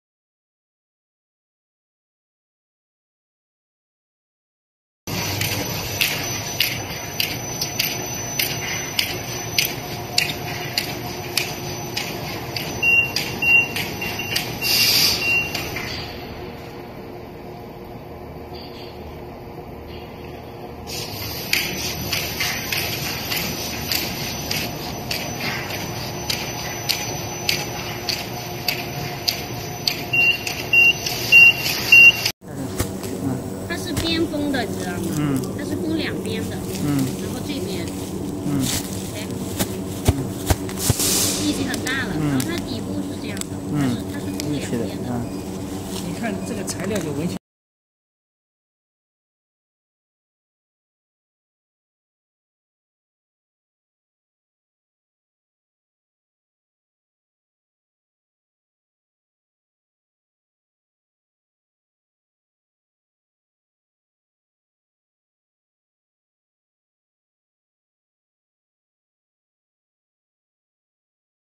Shockproof Air bubble pouch bag making machine
Air bubble bag making machine adopts once-through shaping technique, thus has high efficiency and excellent product quality. This machine set has such features as scientific and reasonable structure design, smooth and reliable running and convenient operation and maintenance.  It not only has good cushioning effect, shock resistance and heat-sealing performance, but also has such advantages as good corrosion resistance and transparency , etc. So it has been widely used for packing, household wares, post parcel, bicycle , polyurethane furniture, etc.

Products Advantages:
1.Top quality
2.Large output
3.Delivery timely
4.Competitive price
5.Satisfying service
6.Old and experienced factory